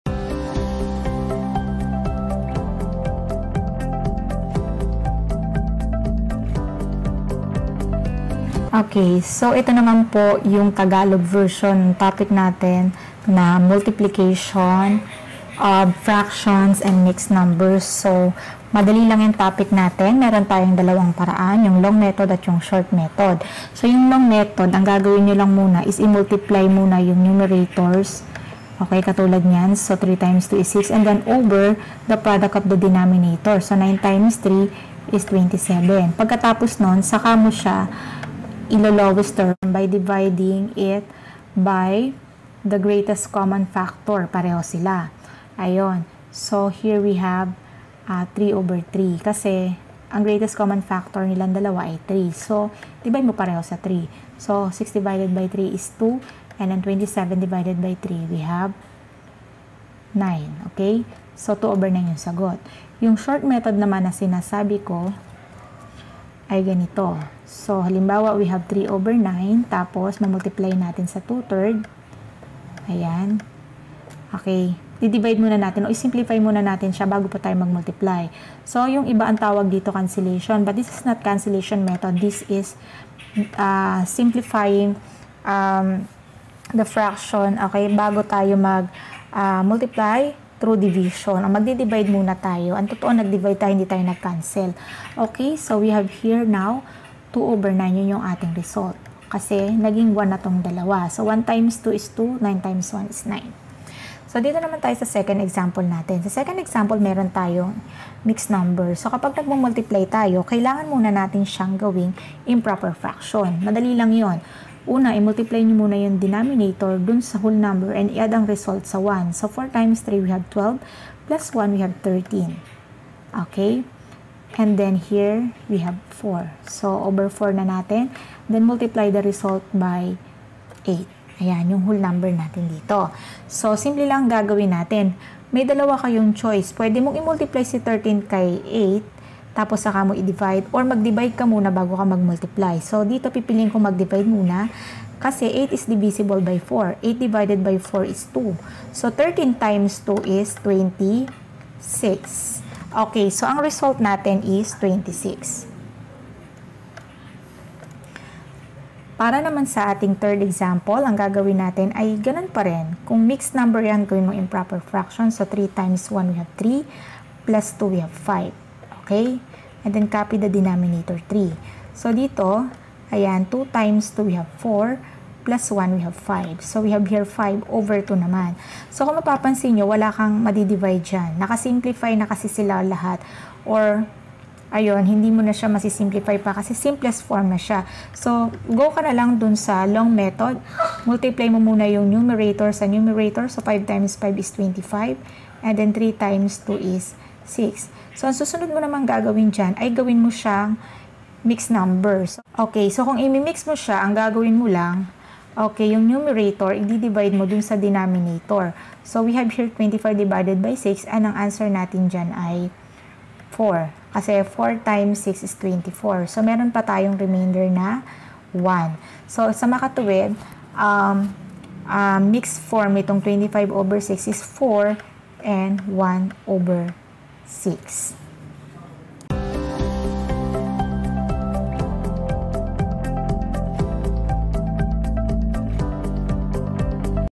PAANO MAGMULTIPLY NG FRACTIONS AT MIXED NUMBERS? (Fun Is Math)
Marunong ka ba mag multiply ng fractions? Eto ang video tutorial na baka makatulong sa'yo kung nahihirapan ka dito!